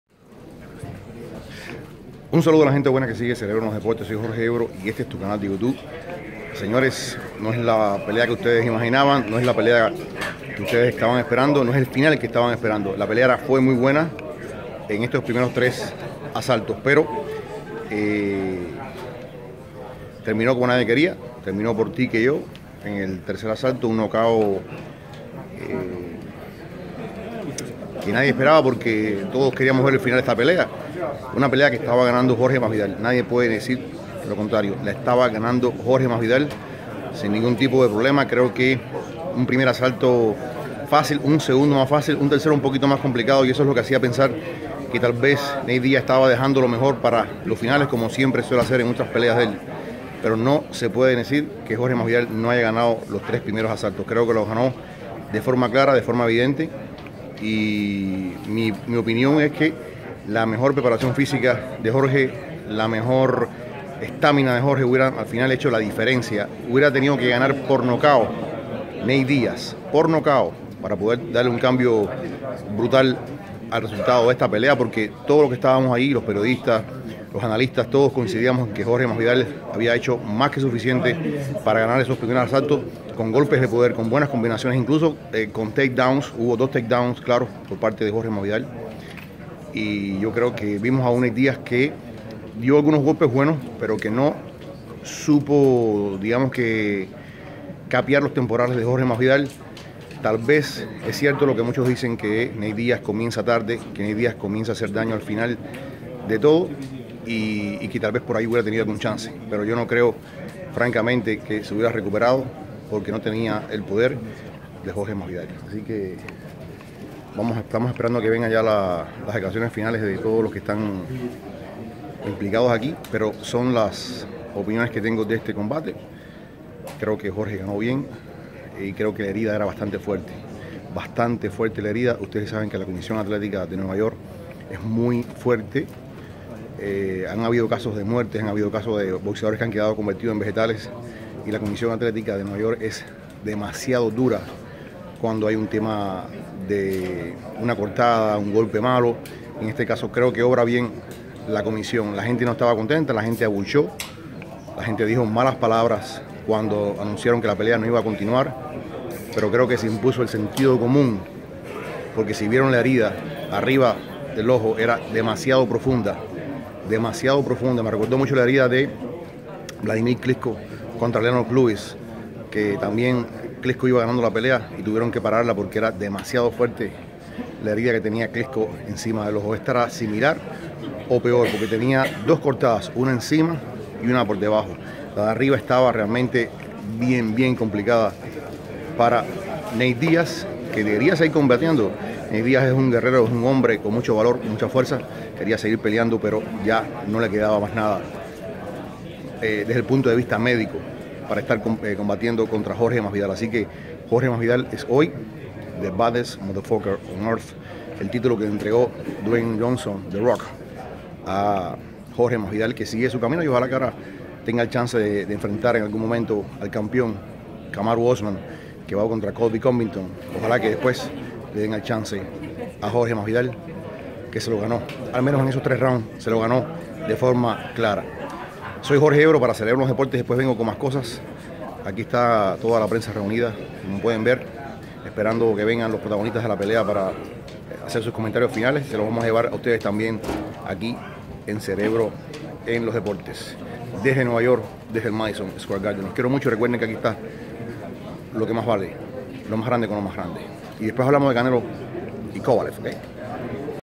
Jorge Masvidal gana el título, pero no como quería
Vence por TKO  a Nate Díaz, luego de que la pelea fuera detenida por grandes cortadas alrededor del ojo derecho